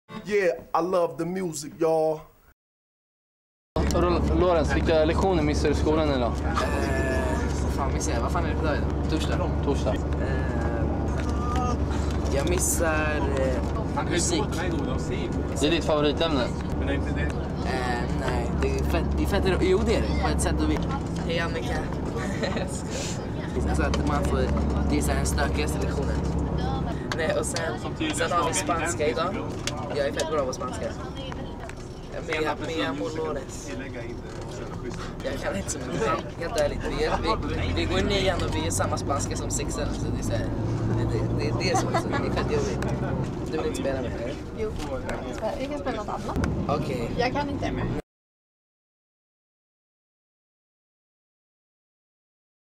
Babyman's Birthday Bus Teaser 4
Lorentz talks about school and which classes he had to cut to be here today